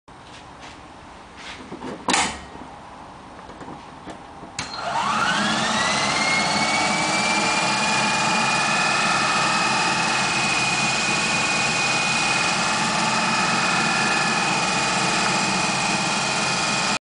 NT Keys Dual AC Compressor PM DC Motor Drive
This is for an all DC solar power system to air condition a house.